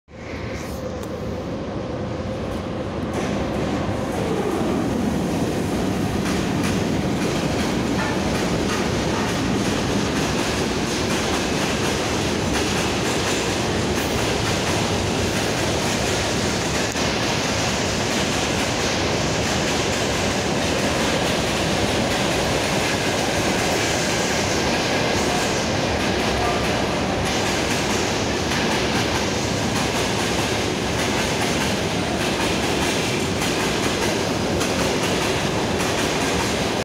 Вл80т-706 пл.Північна №2 перегін Київ Пас-Київ Товарний з думкарною вертушкою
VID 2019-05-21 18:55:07

вл80т-706 пл.Северная №2 перегон Киев Пасс-Киев Товарный с думкарной вертушкой